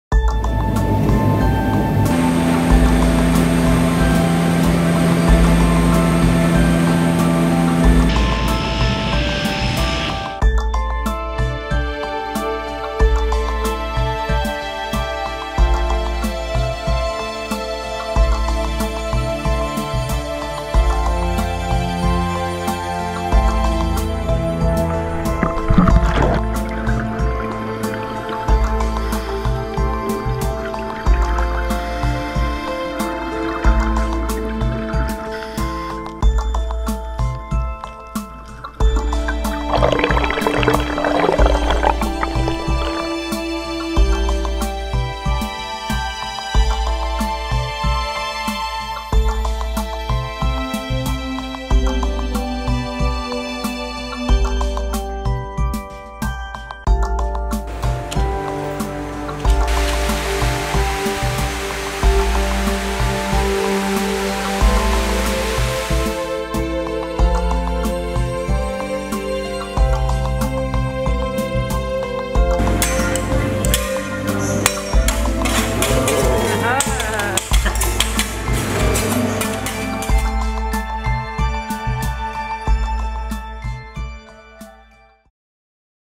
Malediven: Impressionen aus dem Hideaway Beach Resort & Spa
Traumhaftes Luxus-Resort auf der nördlichen Dhonakulhi Island in den Malediven. Wir haben uns umgesehen...